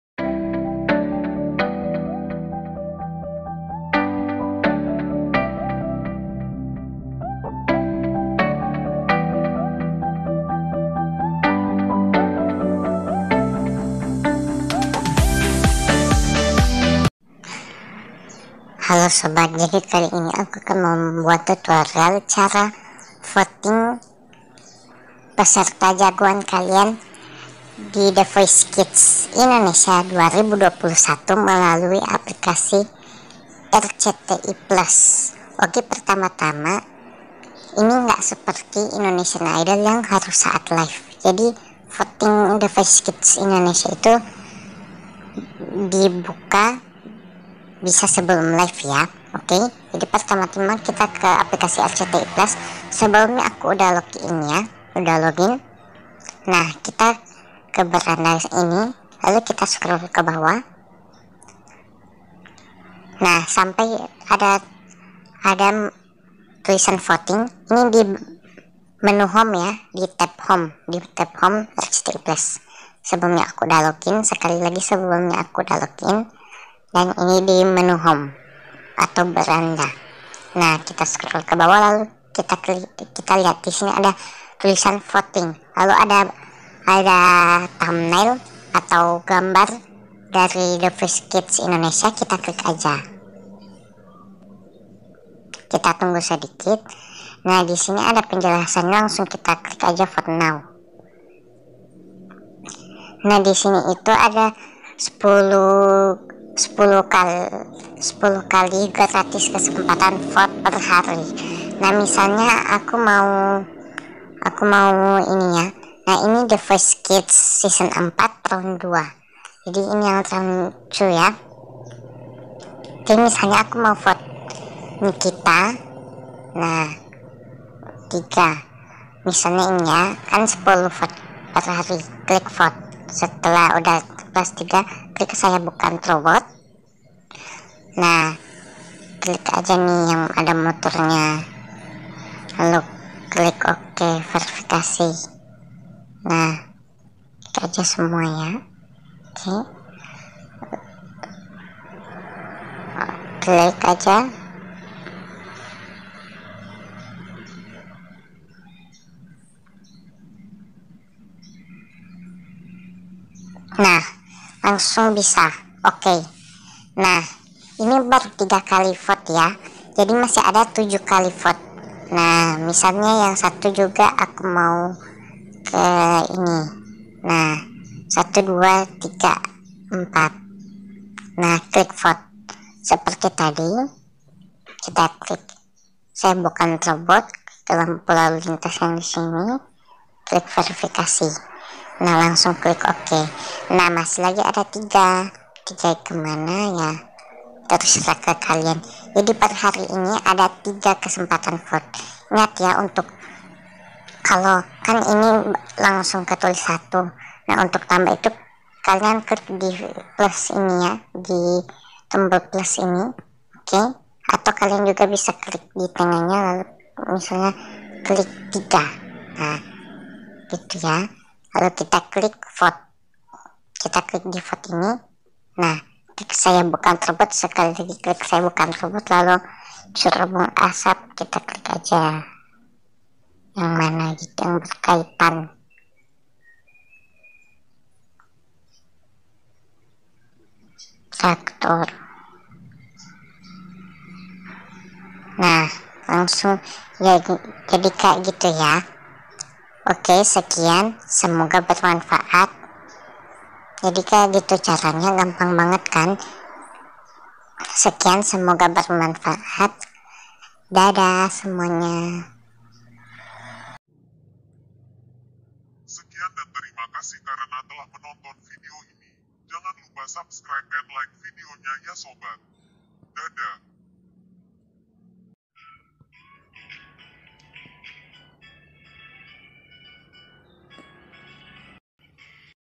Cara Vote Jagoanmu Di The Voice Kids Season 4 menggunakan Aplikasi RCTI+ || cara Vote TVKI 2021
Cara Vote Jagoanmu Di The Voice Kids Season 4 Menggunakan Aplikasi RCTI+ || cara Vote The Voice Kids 2021 Di aplikasi RCTI Plus Terbaru!!